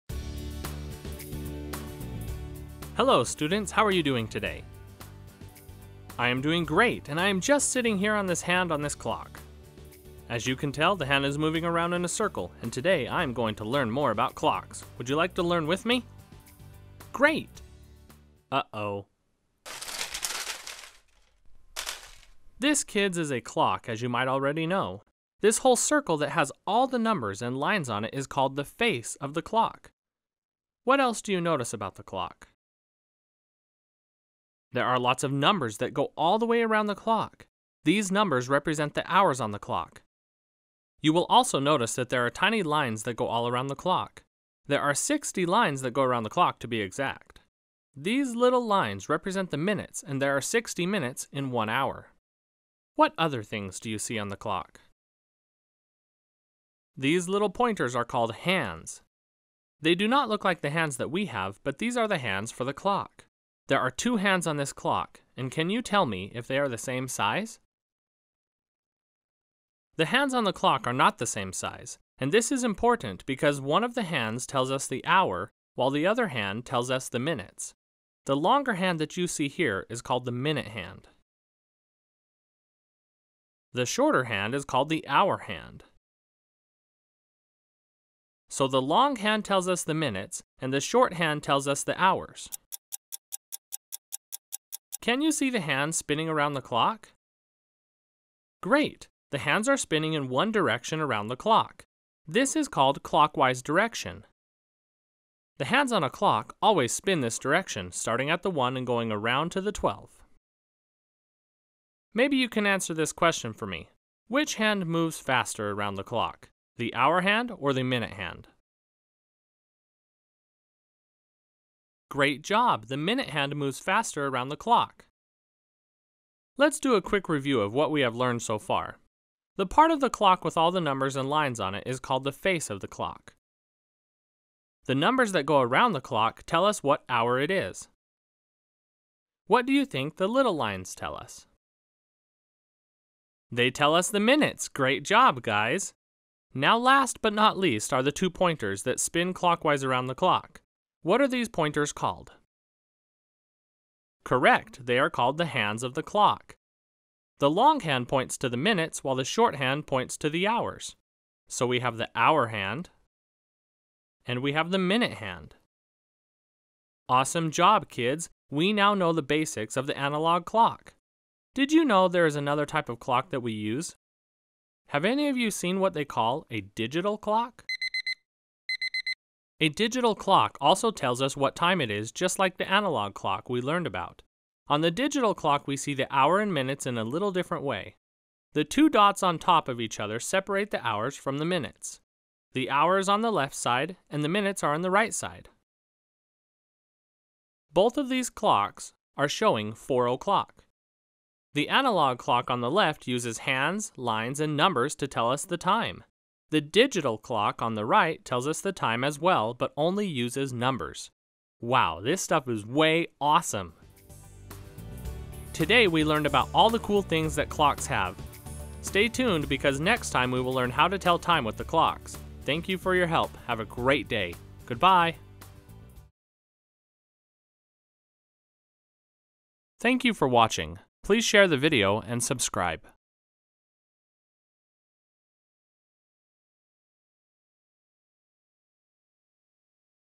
Telling Time - Basics of the Clock 1st Grade
In this video for First Grade we teach the basics of the analog clock and how to tell time. This video goes along with our other videos on telling time and goes along with our math game as well.

Students will learn about the hour hand, minute hand, face of the clock and much more. This video is great for kids in 1st grade, homeschool, and any child that is learning the clock and the common core.